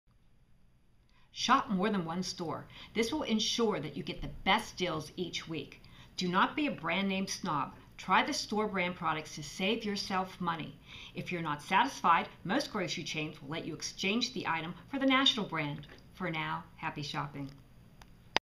Store Brand Savings Tip
Try store brand products. They'll save you lots of money